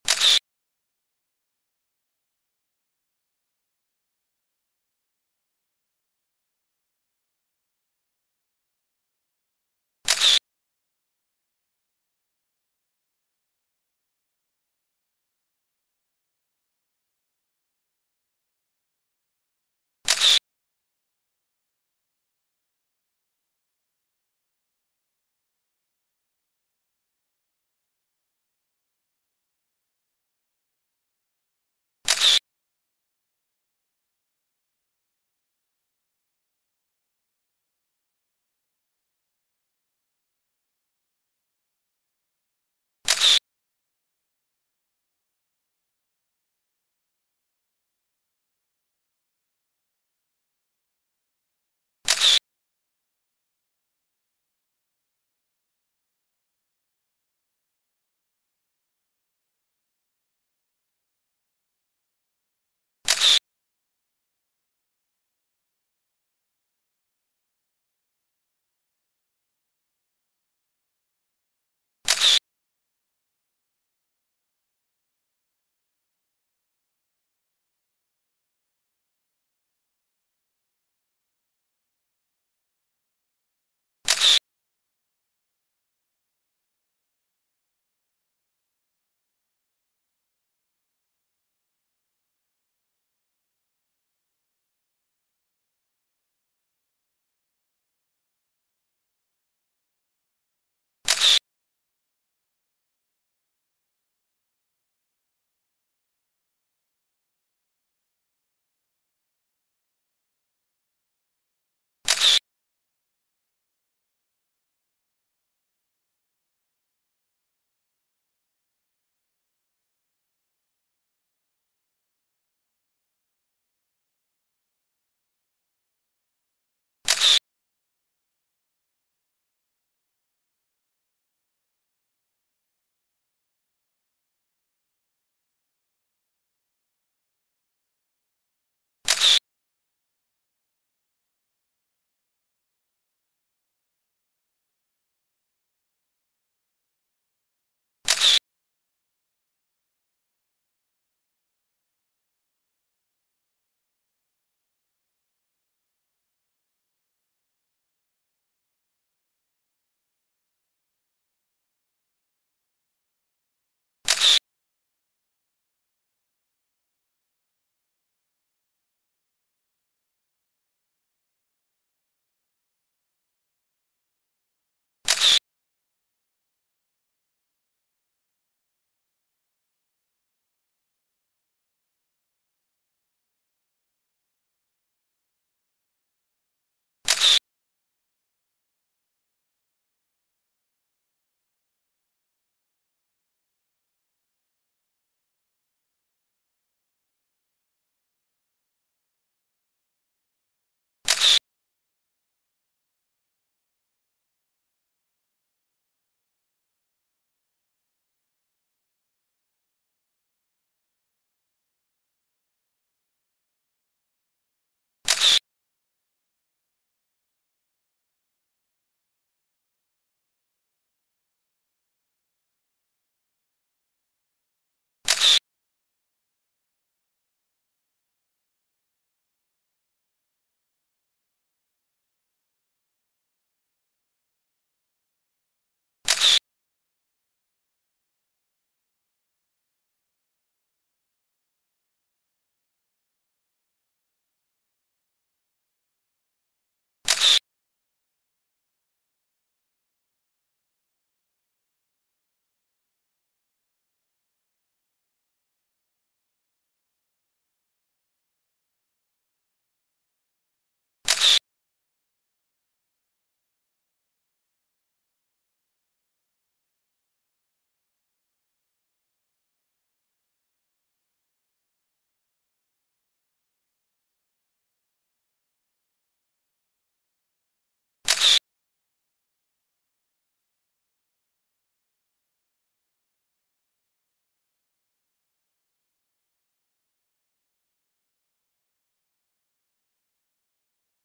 The Day in Sports Photos
CC Sabathia of the Yankees delivers a pitch against the Red Sox on Thursday in New York. The Yankees won 6-2.
The Packers' Aaron Jones celebrates his touchdown against the Rams on Thursday in Green Bay, Wis. The Packers won 24-10.